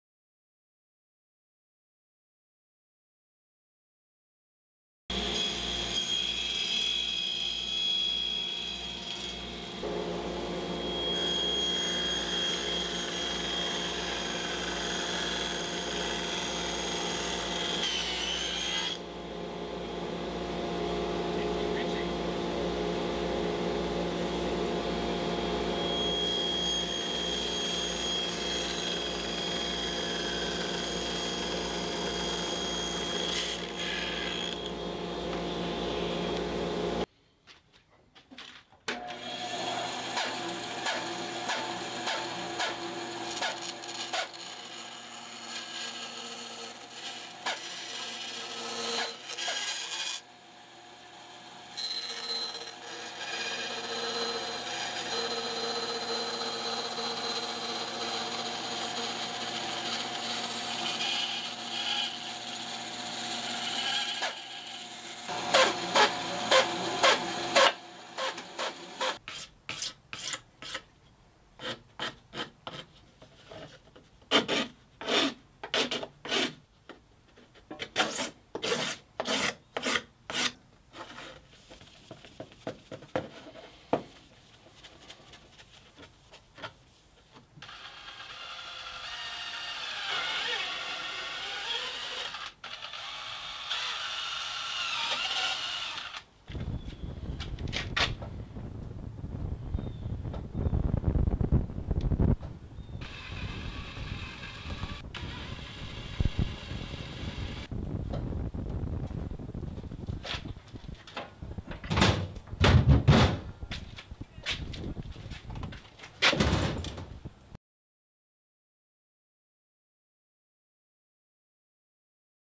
Longboard Holder from PVC Pipe
Simple DIY Longboard holder / hook / shelf made from some 3" PVC.
Made a simple plug/jig for the end to help hold the pipe when cutting.